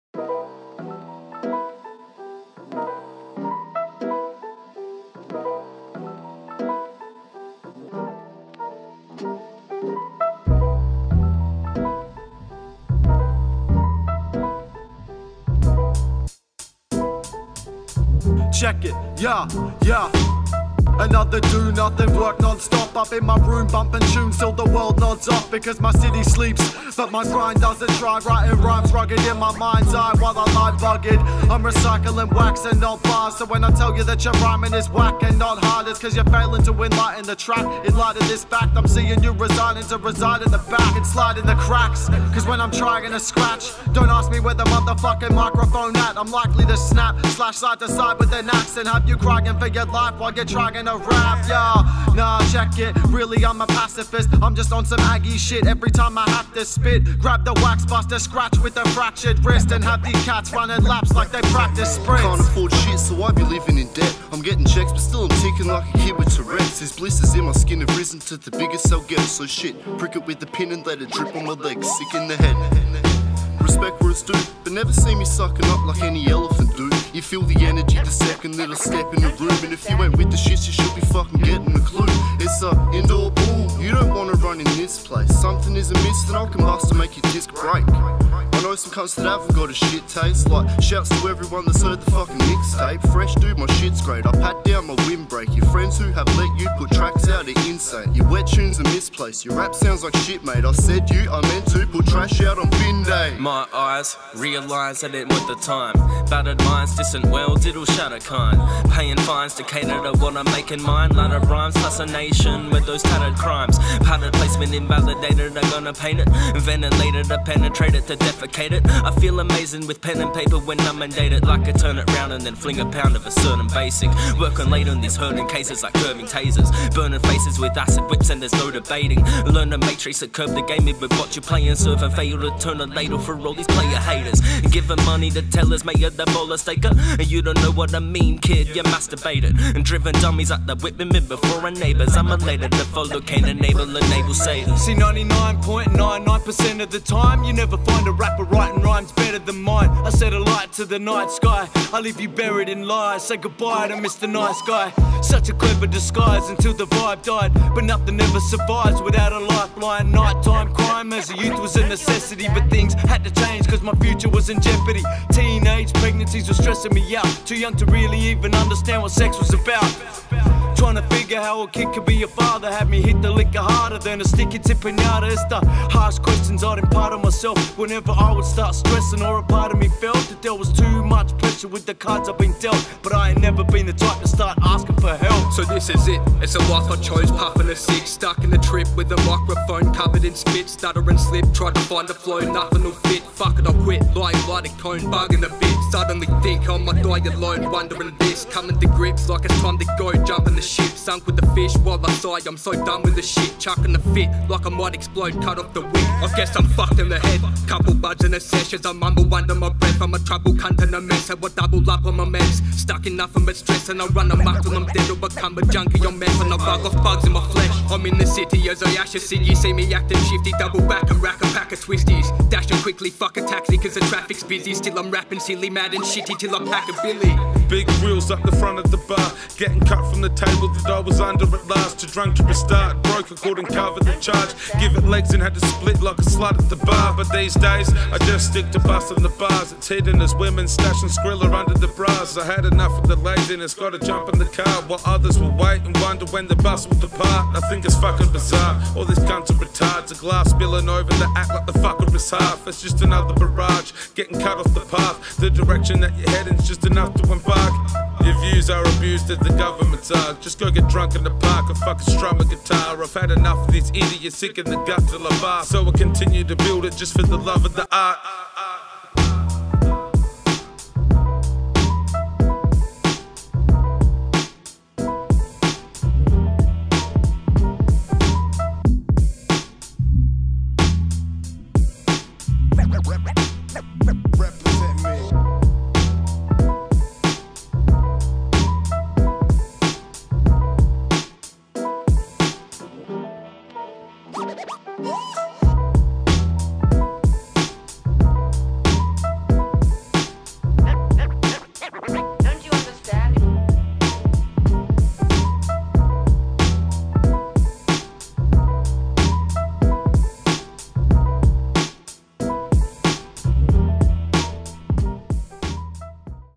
THCtv S3: Cypher Session #1 - NERVE - BUTTER D - NIBS - VOKAL - WOMBAT - GREELEY
THCtv presents Season 3: Cypher Session #1
NERVE - BUTTER D - NIBS - VOKAL - WOMBAT - GREELEY
Produced by Nerve
Recorded, mixed and masted by Nerve at Twisted Records
FILMED BY KYEL GOLLY